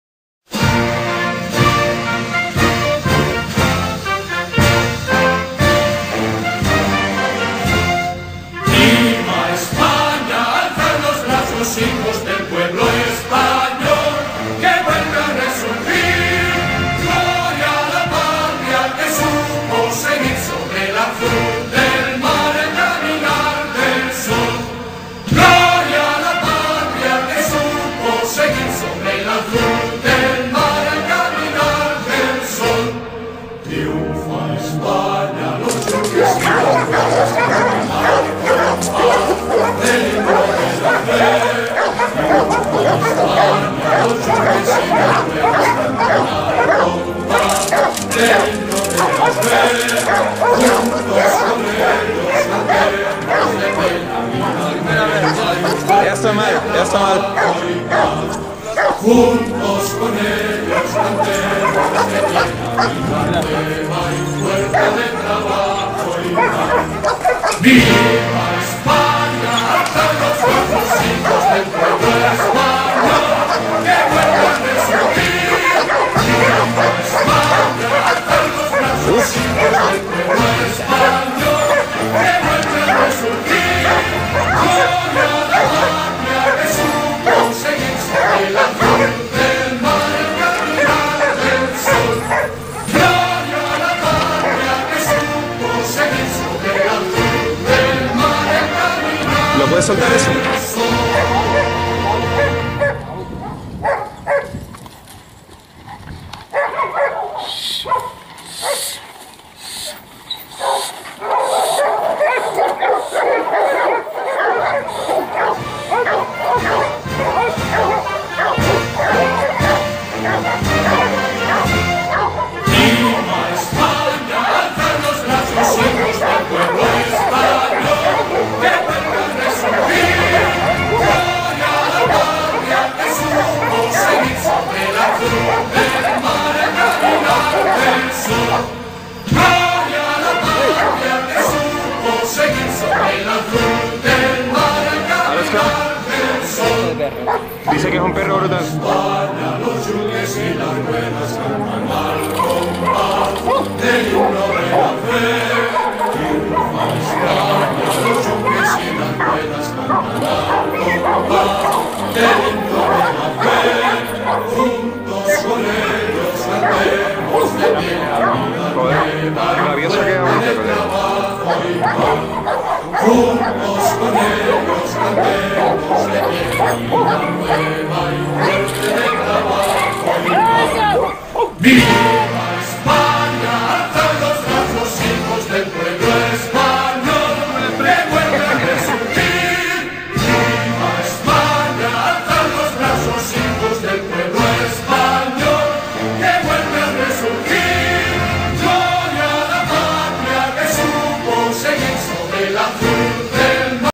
PCE e.V. Atila de Tauco (Import de la Legion ) Perro de Presa Canario
Zu Besuch bei meinen befreudeten Arbeitskennel de Tauco auf Tenneriffa